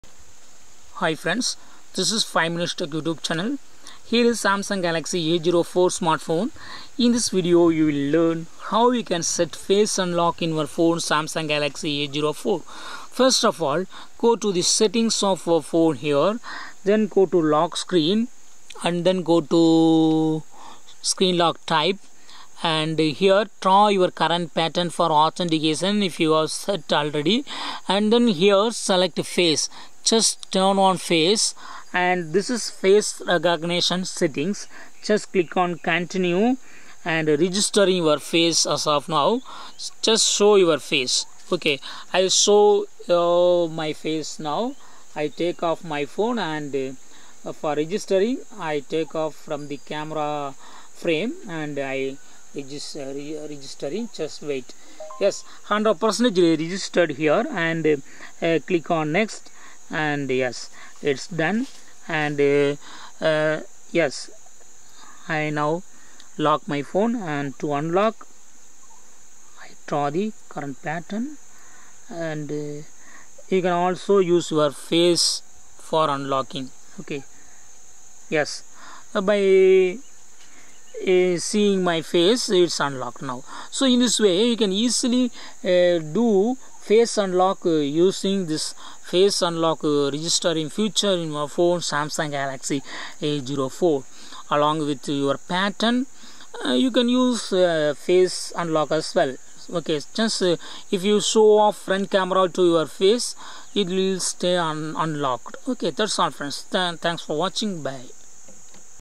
How to set face unlock in Samsung Galaxy A04
Samsung Galaxy A04 face unlock settings: Learn here how to set face unlock in Samsung Galaxy A04 smartphone.